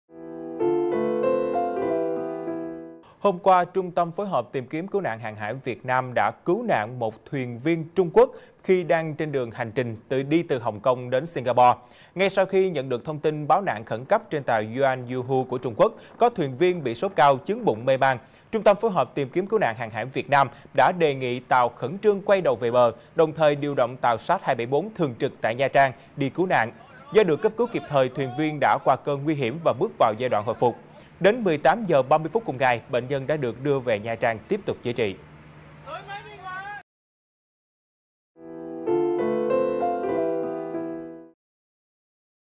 Nóng: Thủy thủ Trung Quốc được Việt Nam cứu nguy | VTC1
VTC1 | Một thuỷ thủ trên tàu hàng của Trung Quốc bị sốt cao, chướng bụng, mê man khi đang trên hành trình qua vùng biển Việt Nam và đã được lực lượng chức năng Việt Nam hỗ trợ.